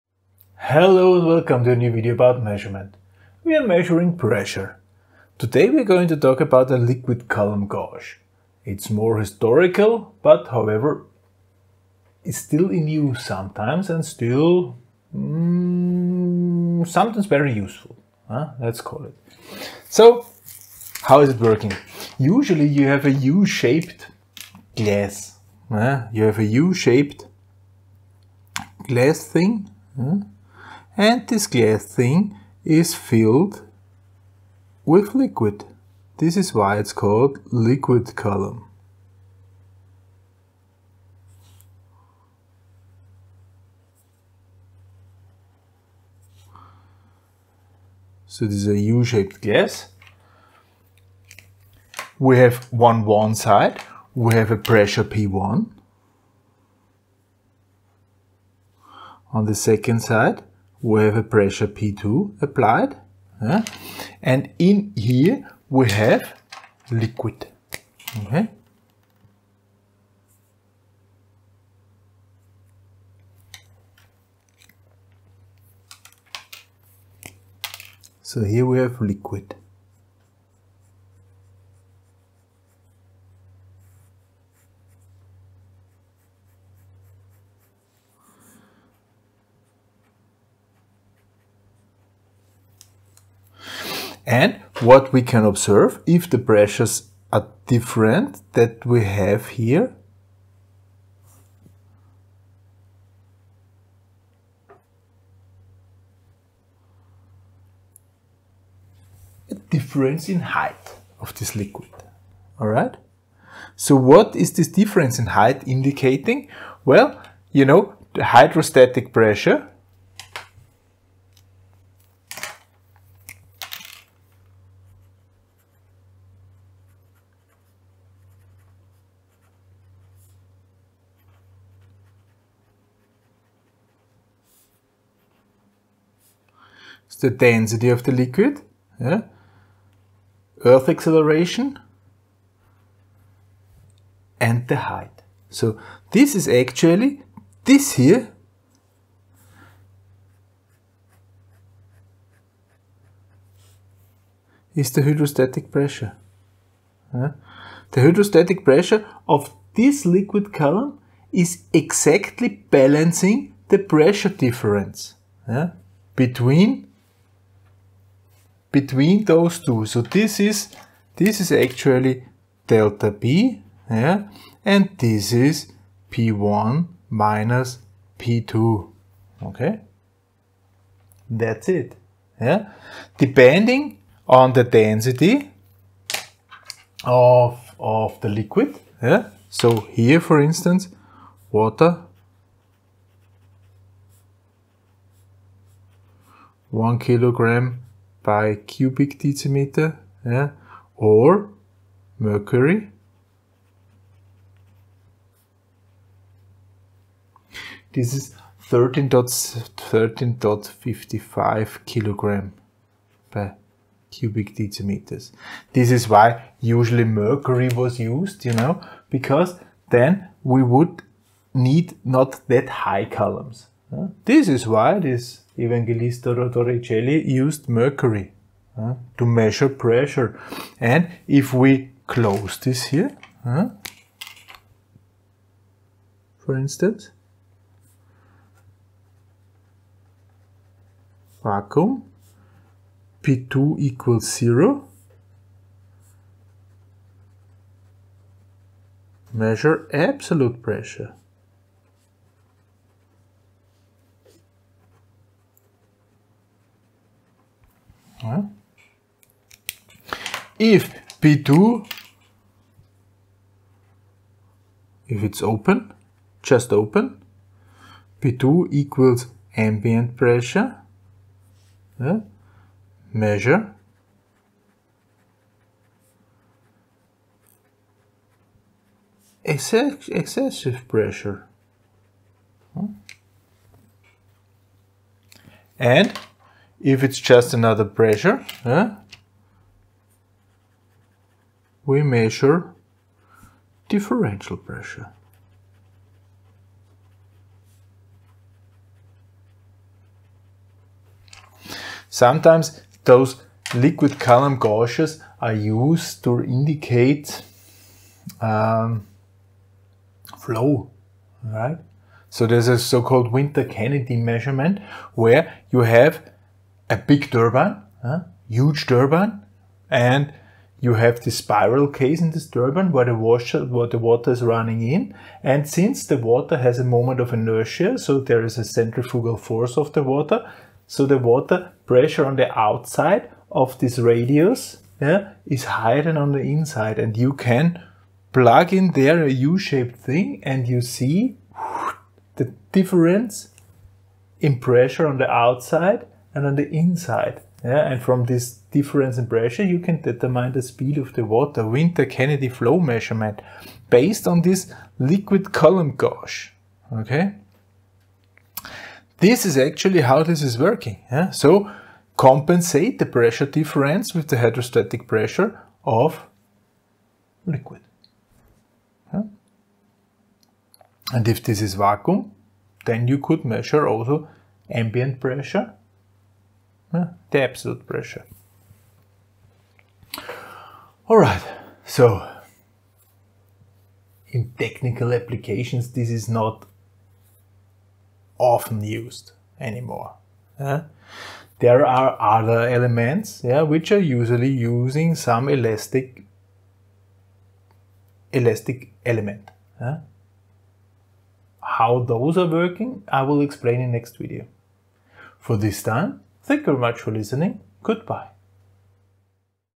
MT 95: Liquid Column Gauge
An old, but still in use methode of determining presures: The liquid column gauge. An explaination of the working principle.